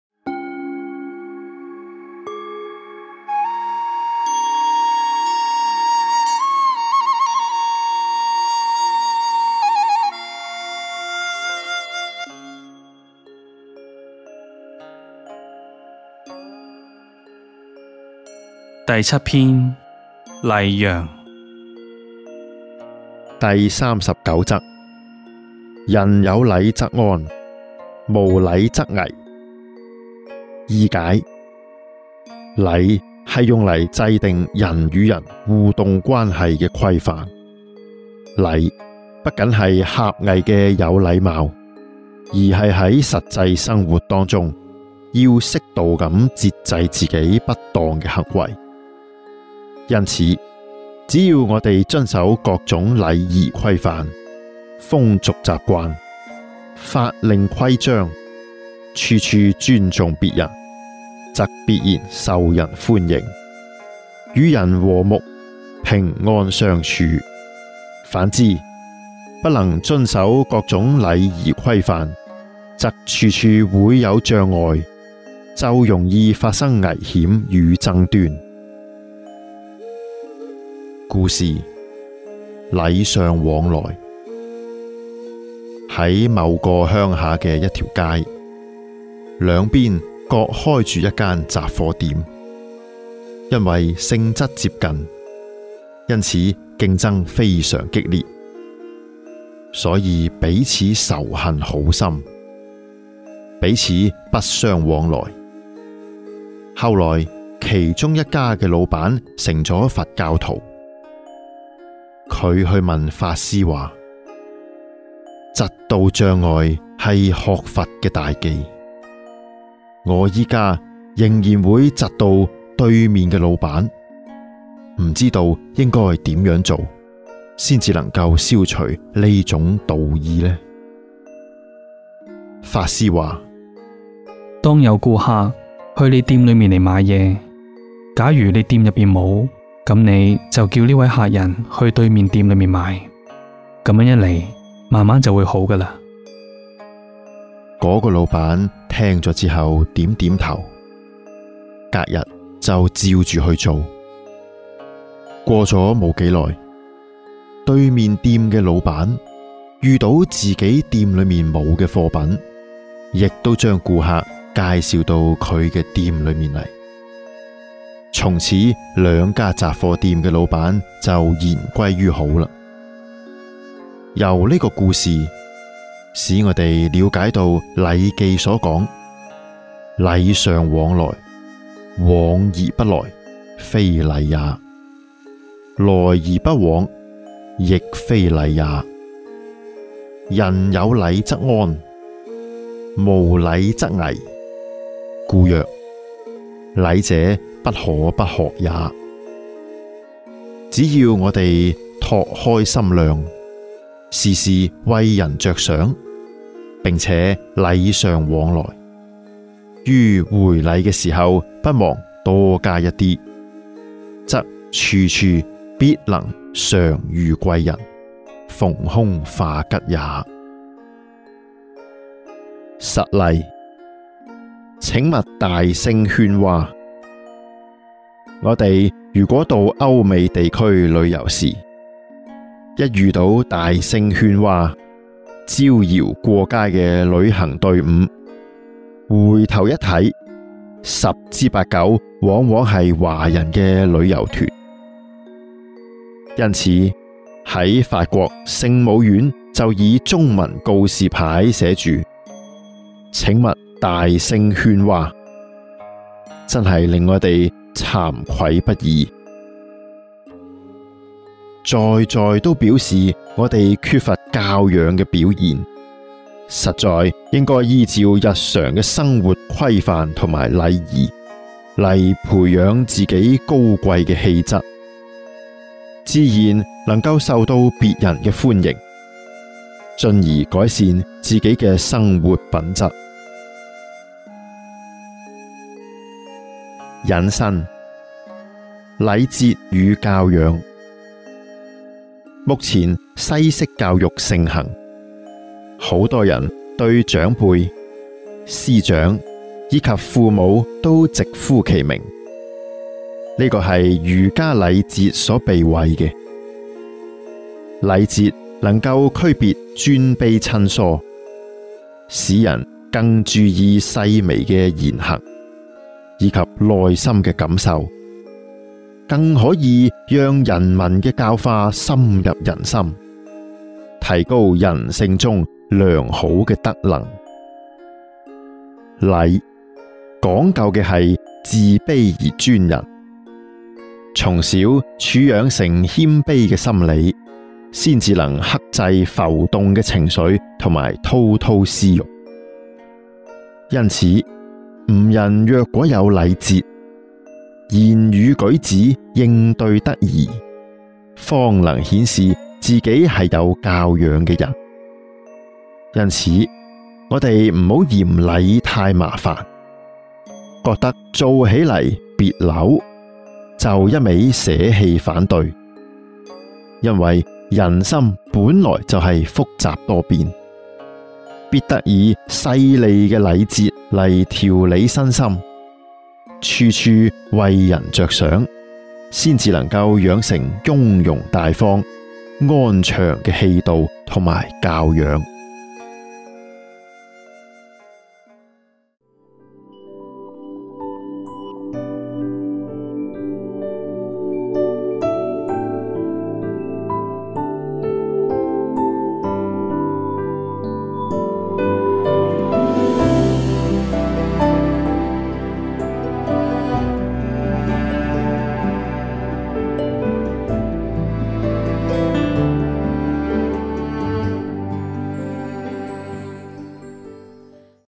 幸福人生六十守則——現代弟子規（粵語版）-第40集
第39則：「人有禮則安，無禮則危。」
【易解】：禮是用來制定人與人互動關係的規範。「禮」不僅是狹隘的「有禮貌」，而是在實際生活當中，要適度的節制自己不當的行為。所以「禮」是規規矩矩的態度。
因此，只要我們遵守各種禮儀規範、風俗習慣、法令規章，處處尊重別人，則必然受人歡迎，與人和睦、平安相處；反之，不能遵守各種禮儀規範，則處處會有障礙，就容易發生危險與爭端。
【故事】:禮尚往來
在某個鄉下的一條街，兩邊各開著一間雜貨店，因為性質接近，因此競爭非常激烈，所以彼此仇恨很深，彼此不相往來。後來，其中一家的老闆成了佛教徒。
他去問法師說：「嫉妒障礙是學佛的大忌。我現在仍會嫉妒對面的老闆，不知該怎麼做，才能消除這種妒意？」
法師說：「當有顧客上你店裡來買東西，假如你店中沒有，那你就叫這客人去對面店裡買。這樣一來，慢慢就會好的。」
那個老闆聽了點點頭，隔天便照著去做。過了不久，對面店裡的老闆，碰到自己店裡沒有的貨品，也把顧客介紹到他店裡來，從此兩家雜貨店的老闆就言歸於好了。
由這個故事，使我們了解到《禮記》所說︰「禮尚往來，往而不來，非禮也；來而不往，亦非禮也。人有禮則安，無禮則危。故曰，禮者不可不學也。」只要我們拓開心量，時時為人著想，並且禮尚往來，於回禮時不忘多加一點，則處處必能常遇貴人，逢凶化吉也。

【實例】：「請勿大聲喧嘩」
我們若到歐美地區旅遊時，一遇到大聲喧嘩、招搖過街的旅行隊伍，回頭一看，十之八九往往是華人的旅遊團。
因此，在法國聖母院就以中文告示牌書寫：「請勿大聲喧嘩」，真令我們慚愧不已。在在都表示我們缺乏教養的表現，實應依照日常的生活規範與禮儀，來培養自己高貴的「氣質」，自然能受到別人的歡迎，進而改善自己的生活品質。
【引申】：禮節與教養
目前西式教育盛行，很多人對長輩、師長及父母都直呼其名，這是儒家禮節所避諱的。「禮節」能區別尊卑親疏，使人更注意細微的言行，及內心的感受，更可以讓人文的教化深入人心，提高人性中良好的德能。
「禮」，講究的是「自卑而尊人」，從小處養成謙卑的心理，才能克制浮動的情緒和滔滔私欲。
因此，吾人若有「禮節」，言語舉止應對得宜，方能顯示自己是有「教養」的人。因此我們不要嫌「禮」太麻煩，覺得做起來彆扭，就一味捨棄反對。
因為人心本來就複雜多變，必得以細膩的禮節來調理身心，處處為人著想，才能養成雍容大方，安詳的氣度與教養。